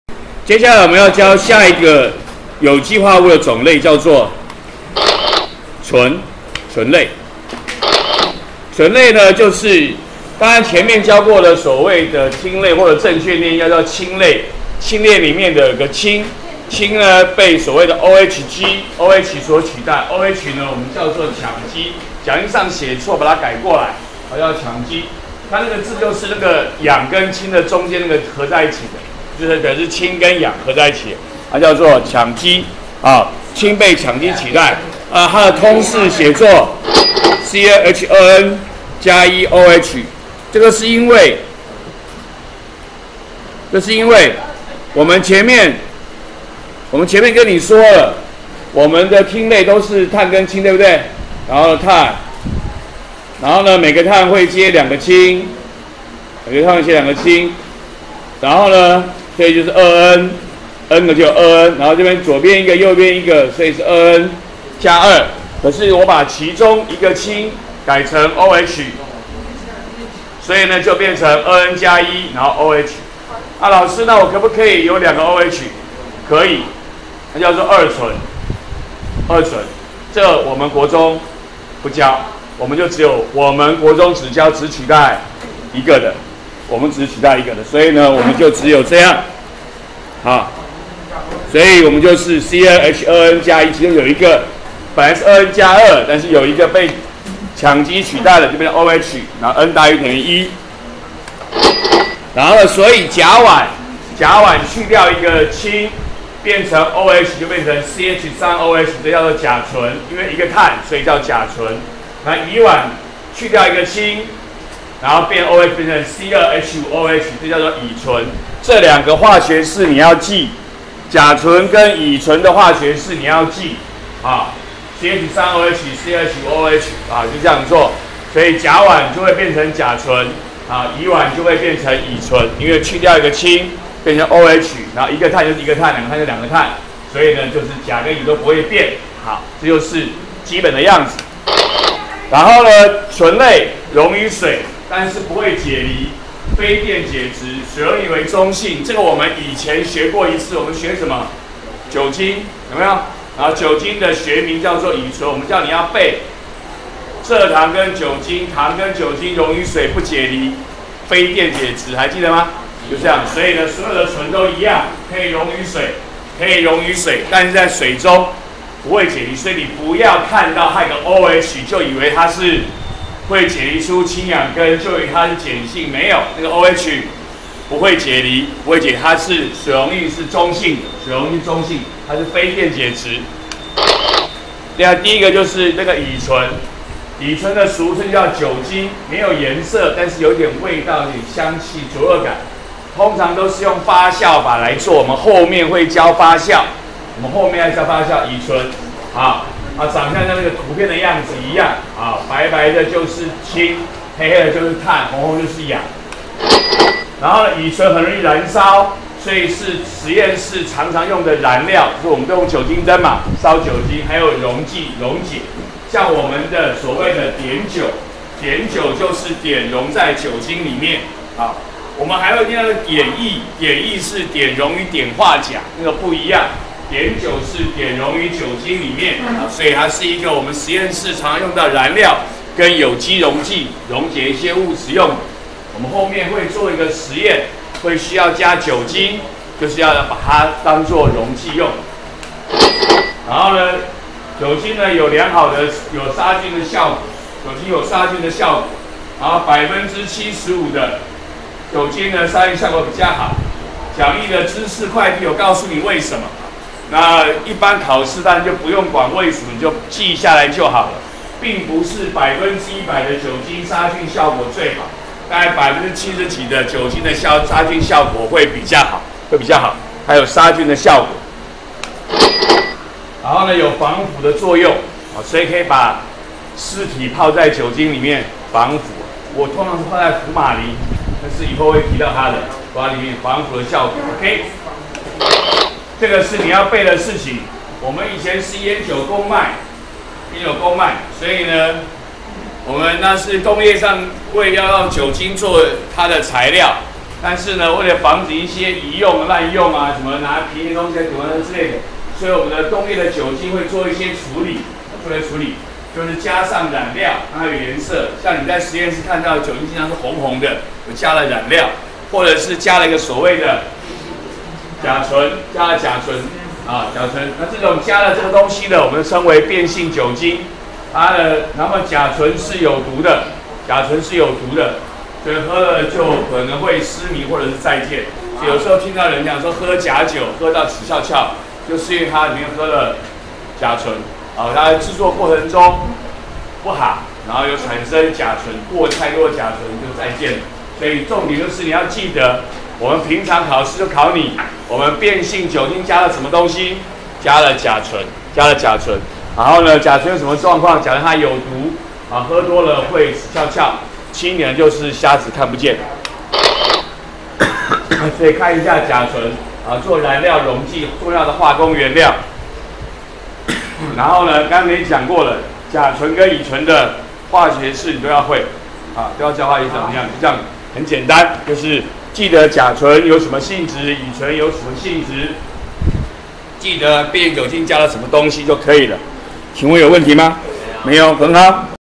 102學年國二理化_有機化合物05：醇類
介紹醇類，說明何謂醇類、醇類官能基，醇類性質，介紹甲醇、乙醇及變性酒精。